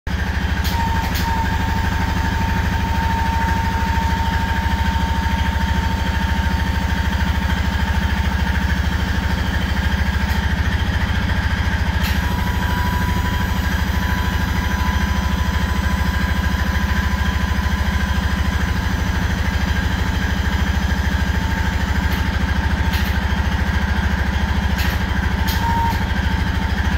Warn atv plow hydraulic actuator modification Honda Rubicon 520
testing out my addition of a hydraulic actuator on my warn plow.  Honda rubicon 520